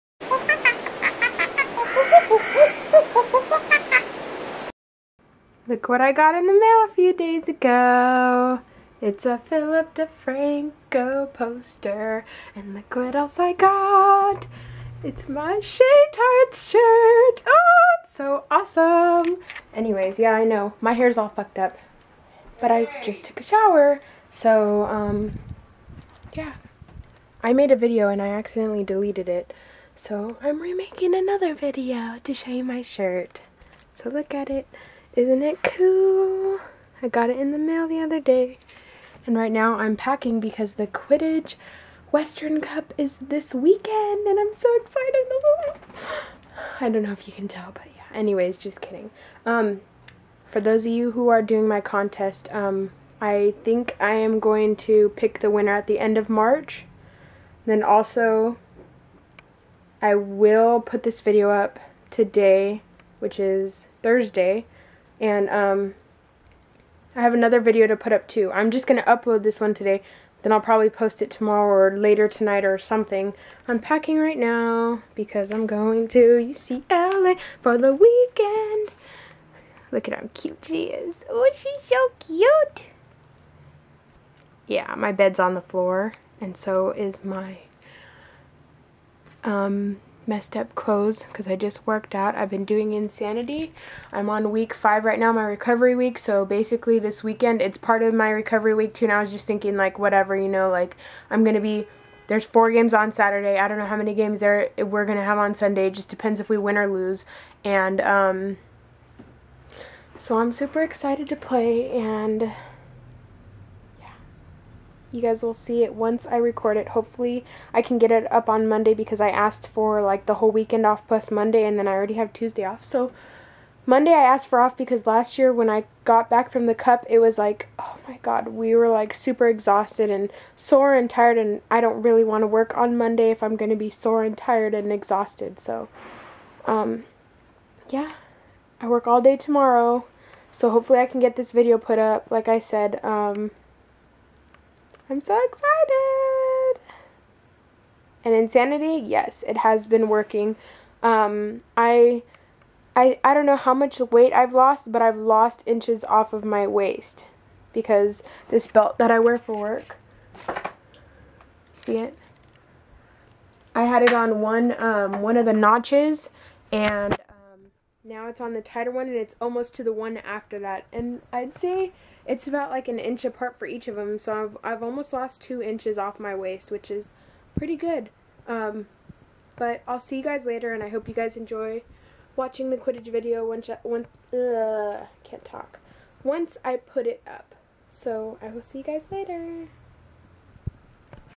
Amanda Is Super Excited For The Weekend!
Amanda shows her PhillipDeFranco signed poster and her 3 year anniversary Shaytards shirt which is awesome! She also talks about how excited she is for the Western Cup Quidditch Tournament and that she will be choosing the winner of the calendar at the end of March. She talks a little about Insanity and that she is on week 5 which is her recovery week.